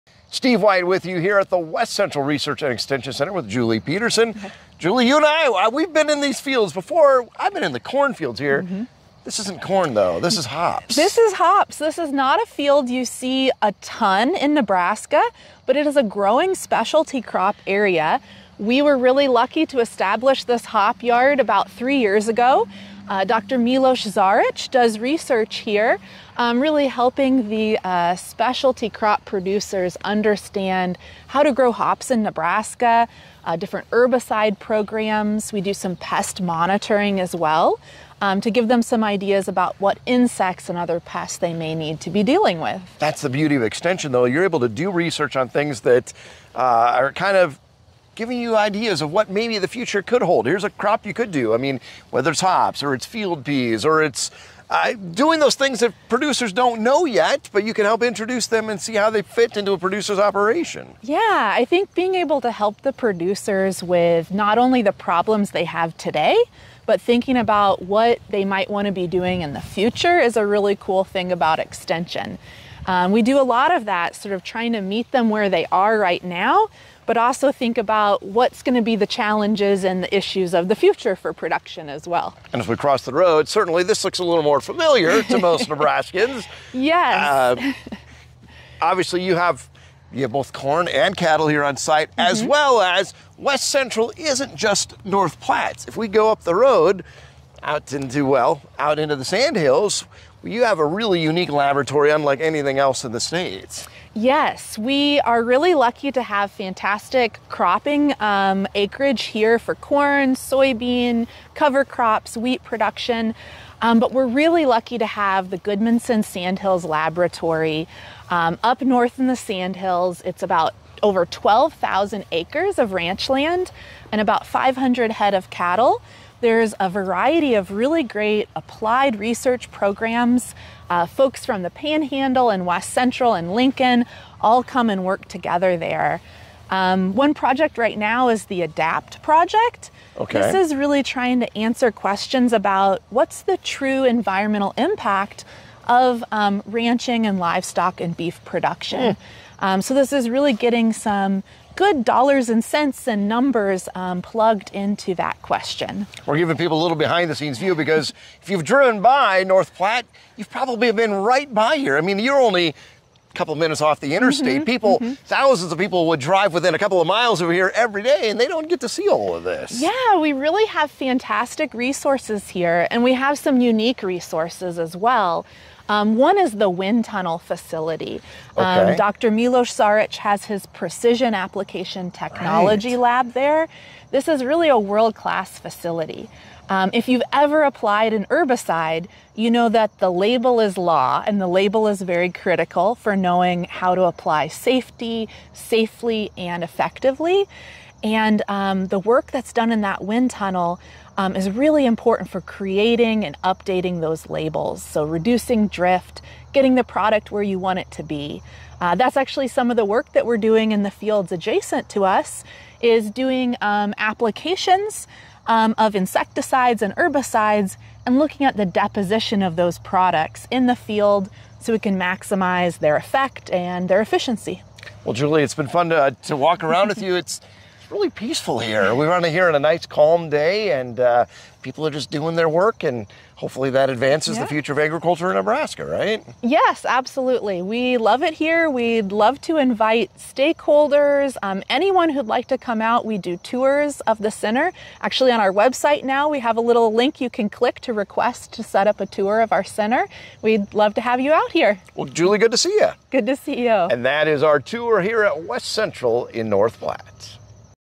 Hops, Corn, Cattle and Much More! | Research at WCREEC | RRN Roadshow
The Rural Radio Network Roadshow stops at the West Central Research, Extension & Education Center in North Platte! 🚜

Interim Director Julie Peterson gives us a behind-the-scenes look at the innovative research happening here:

🌱 Specialty crop trials, including hops and field peas

🌾 Corn, soybean, wheat, and cover crop studies

🐂 The Goodmanson Sandhills Lab with 12,000 acres and 500 head of cattle

🌬️ A world-class wind tunnel facility for pesticide and herbicide application research

These programs are shaping the future of Nebraska agriculture by solving today’s problems and preparing for tomorrow’s challenges.

📍 Tours are available! Visit WCREEC online to learn more.